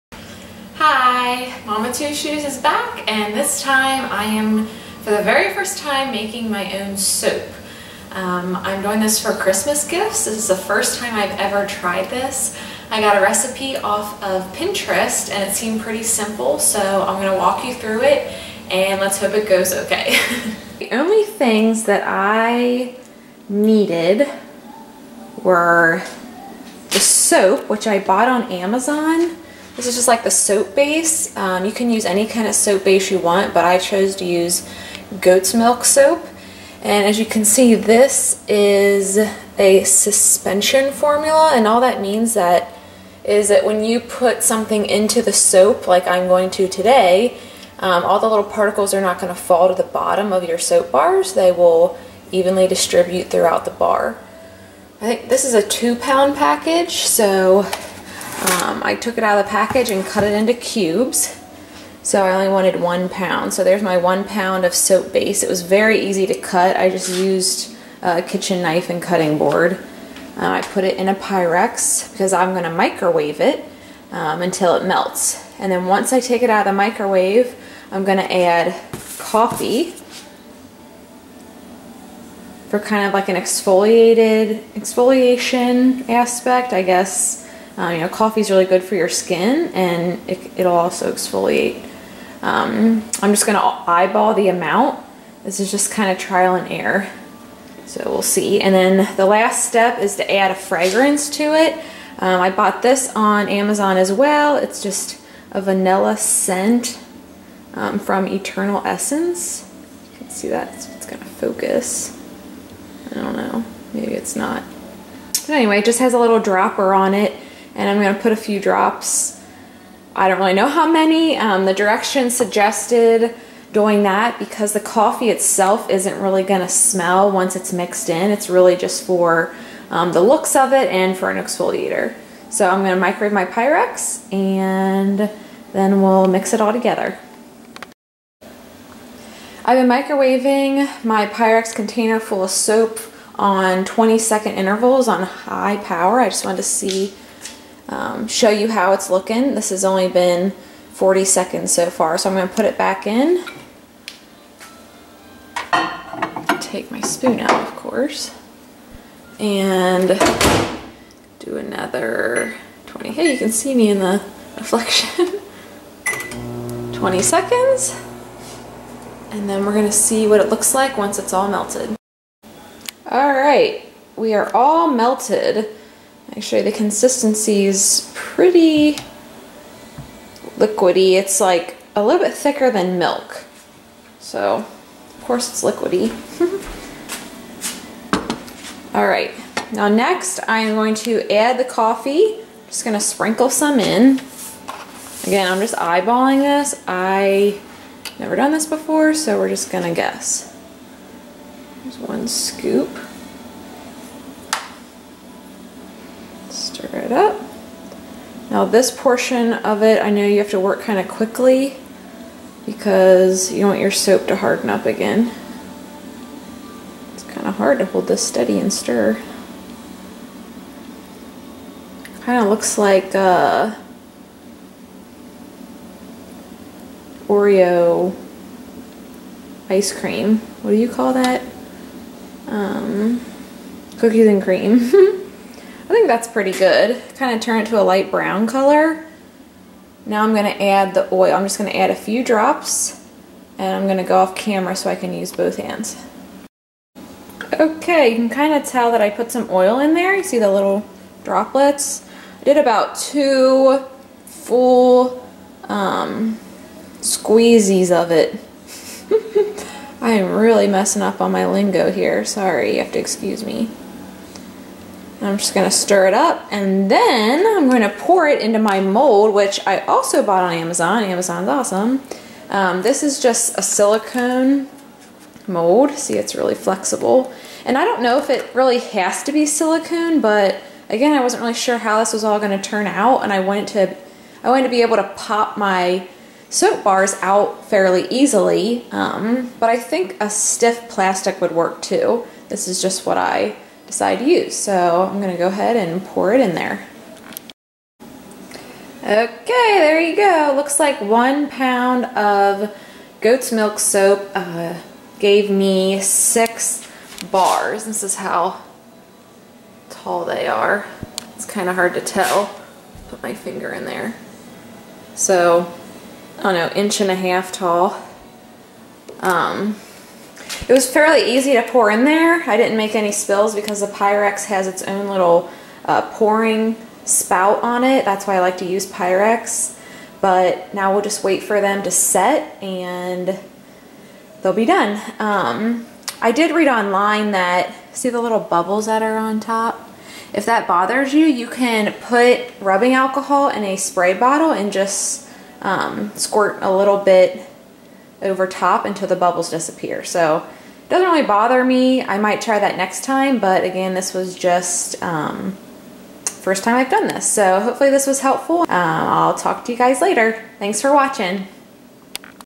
Vanilla Coffee Soap DIY easy
Watch me make soap for the first time on camera! This would be a great homemade gift for someone :)
Here is the soap mold used
Here is the vanilla scent